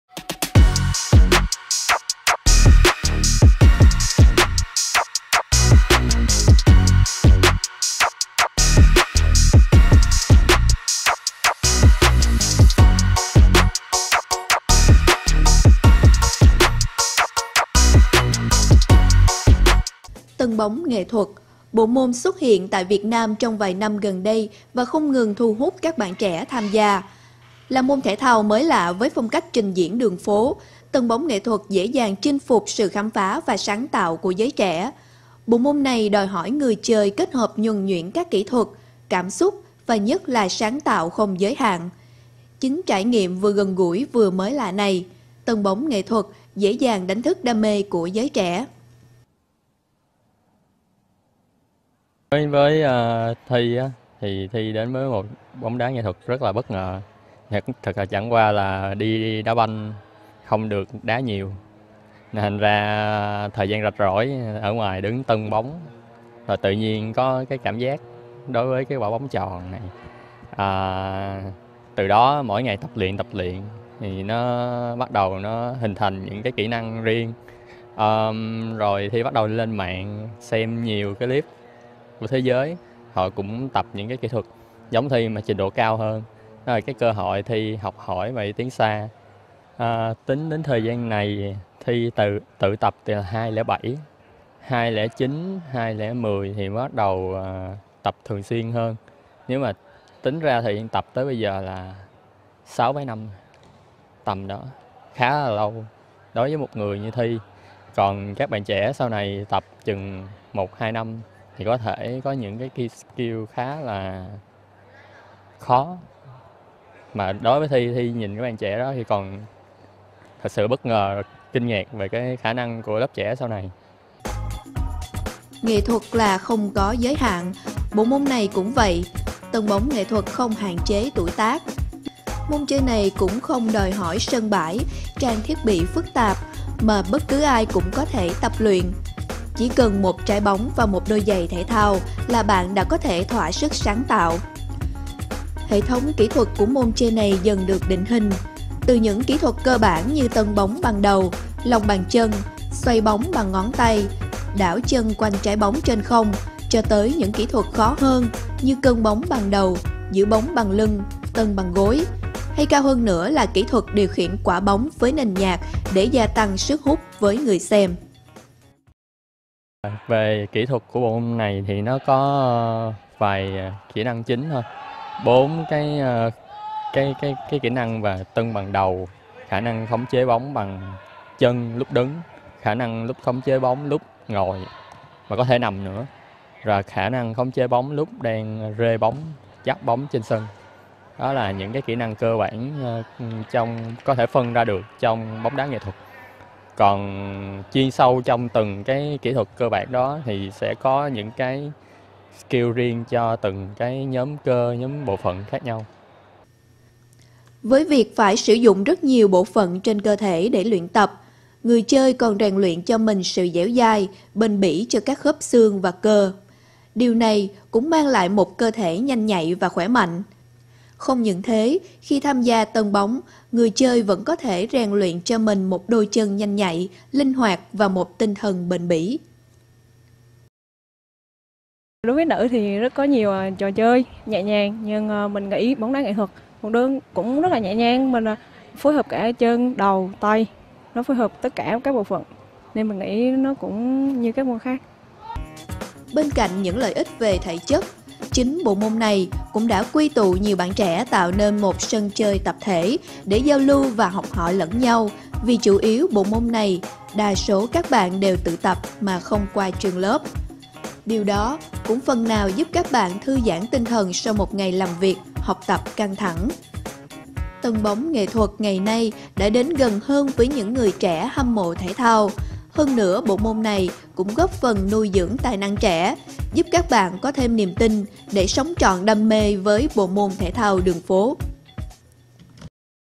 THY FREESTYLE giới thiệu về Tâng Bóng Nghệ Thuật trên sóng truyền hình HTV7 (2015)
Lúc này là năm 2015, thời mà Thy còn trẻ trung lần đầu làm nhân vật chính trong một phóng sự của Đài Truyền Hình HTV7 bộ môn Tâng Bóng Nghệ Thuật

THY FREESTYLE giới thiệu về Tâng Bóng Nghệ Thuật trên sóng truyền hình HTV7